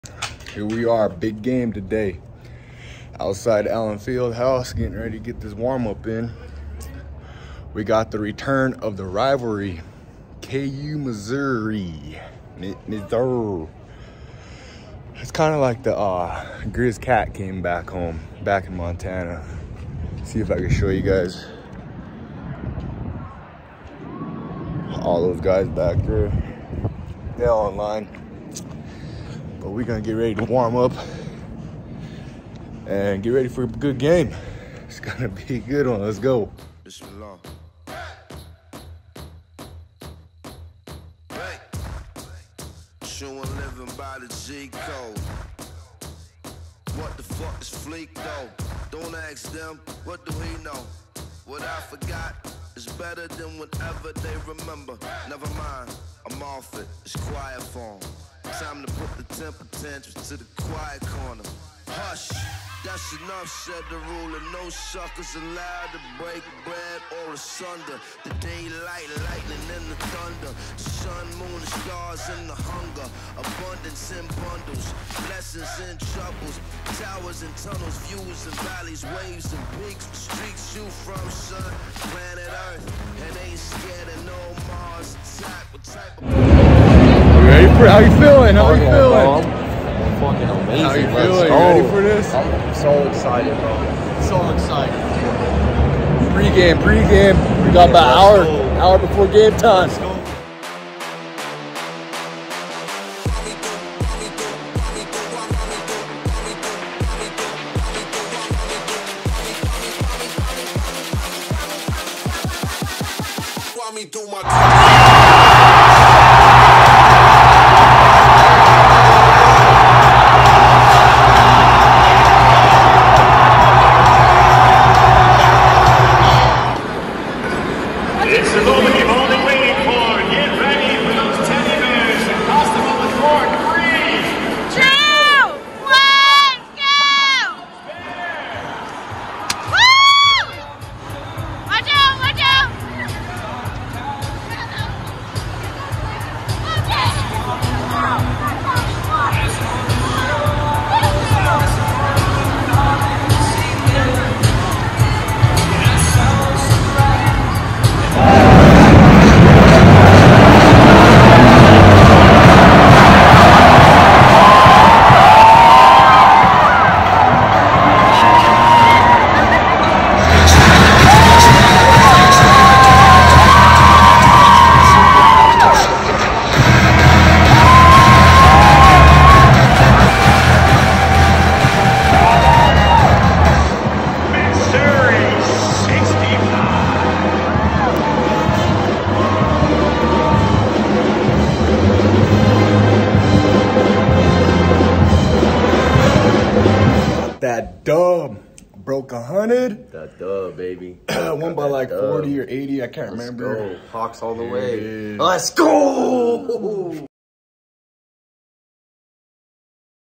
KU Cheerleading: Allen Fieldhouse (Indigenous Edition)
Indigenous Student-Athlete Cheerleader, Myltin Bighorn (Fort Peck Sioux), takes you through a KU Men’s Basketball game in historic Allen Fieldhouse.